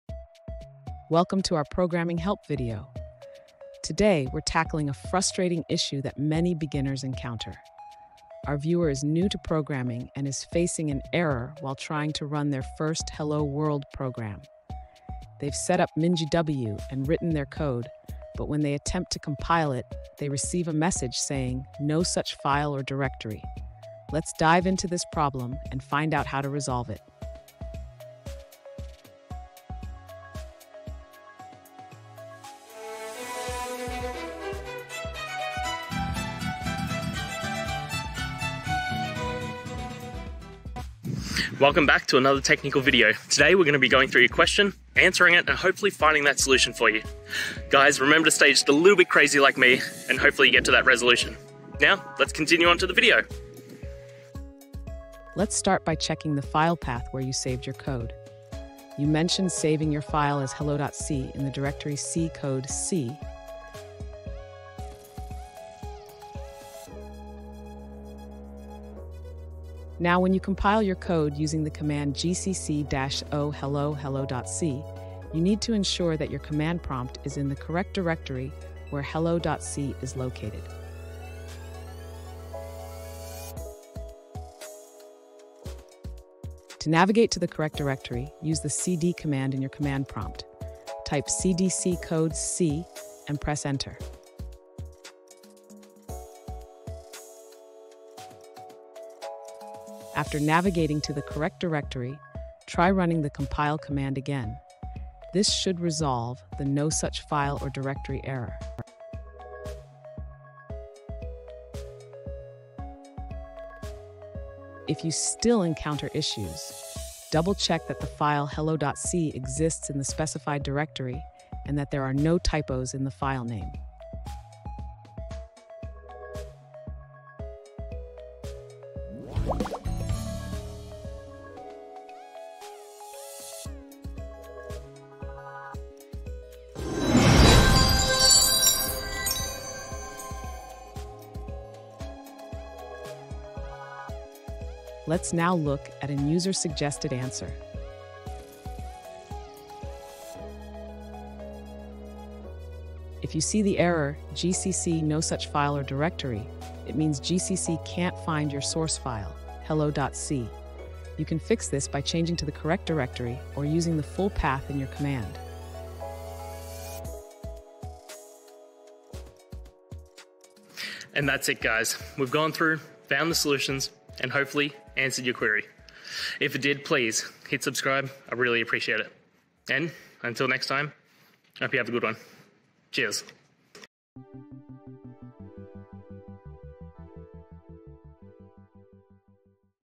Fixing GCC Error: No Such File or Directory - Troubleshooting Guide
In this video, we tackle a common issue faced by developers when working with the GCC compiler: the dreaded "No Such File or Directory" error. Whether you're compiling C or C++ code, this error can be frustrating and time-consuming. Join us as we walk through the troubleshooting steps to identify the root cause and provide effective solutions to get your code compiling smoothly again.

Today's Topic: Fixing GCC Error: No Such File or Directory - Troubleshooting Guide

Related to: gccerror, nosuchfileordirectory, troubleshootingguide, gcctroubleshooting, fixgccerror, programmingerrors, compilererrors, cprogramming, c++programming,